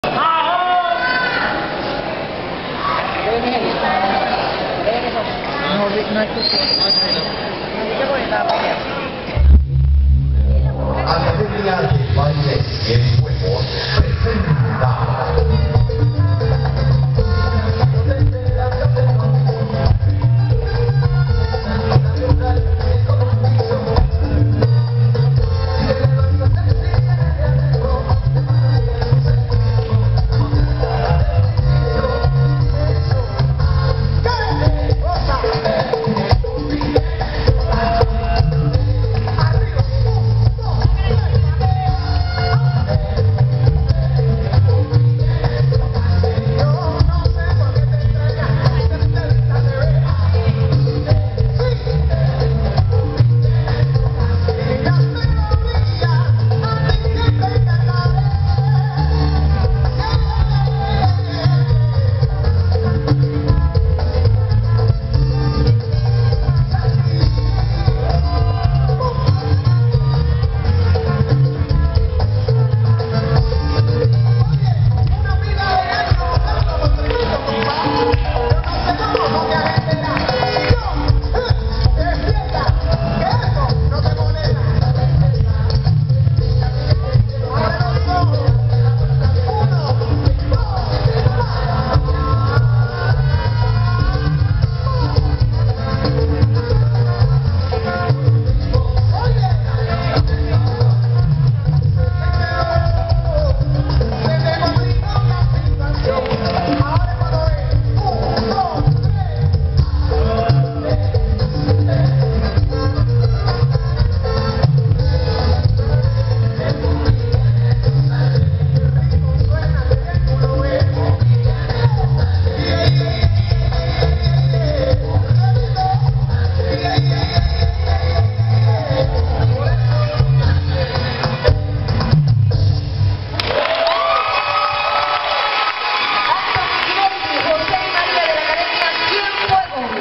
SALSA CASINO - Dueto de CienFuegos #3 (Dueto de Parejas)
Integrantes: Antony Grimaldi, Mileily Salgado, Jose Cepeda y Maria Arambulo
Lugar: 10°
Academia: CienFuegos

¡Bailar es Solo el Comienzo!